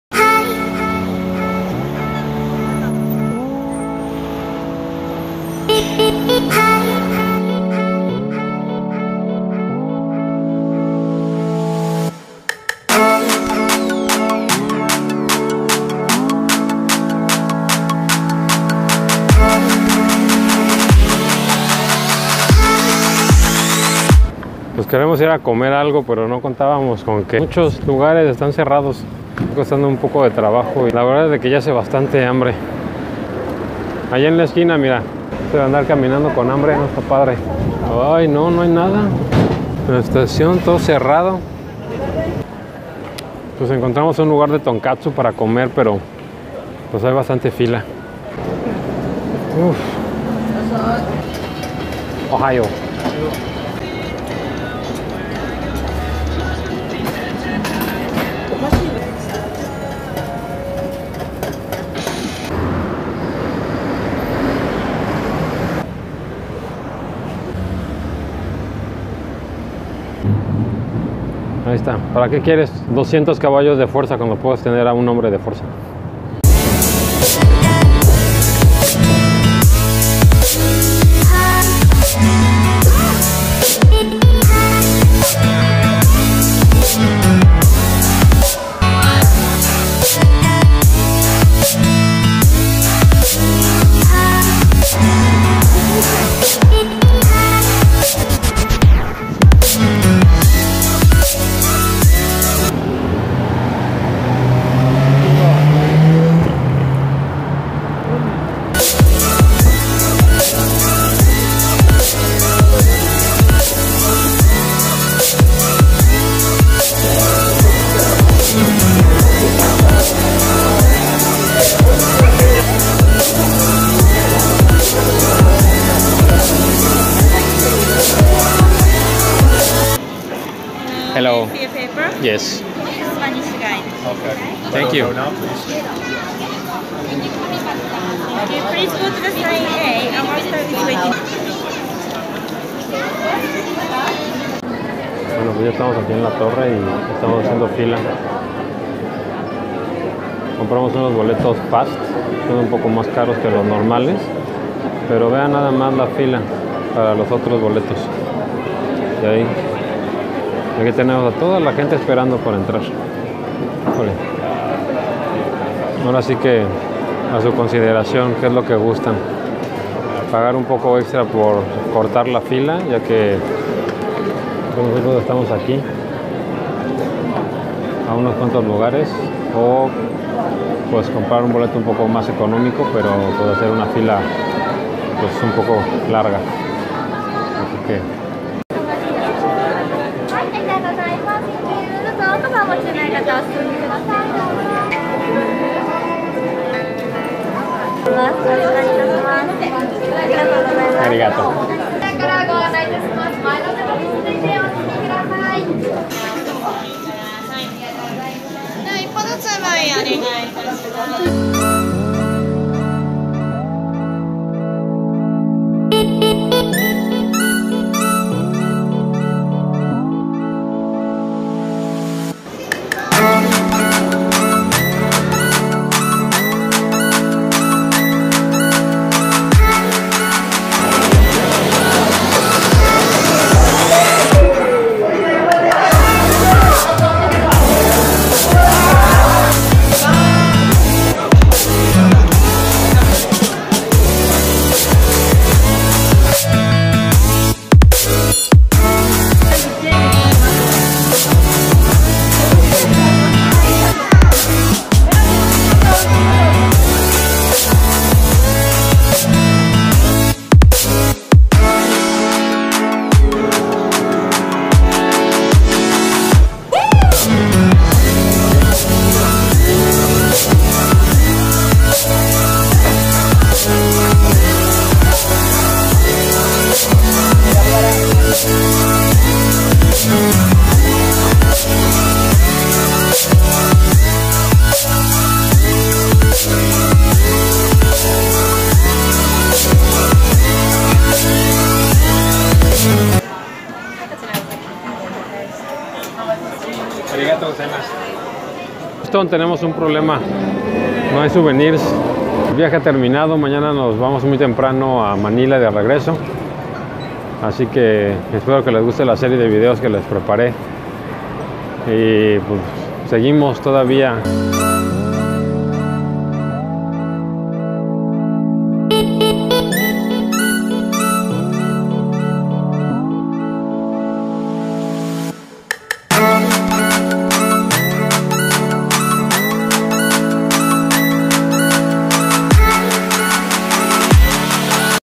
Viaje a Japon (Tokyo) - La MEJOR vista desde el Sky Tree
La mejor vista de Tokyo, desde el Sky Tree. Nuestro último destino en Japón.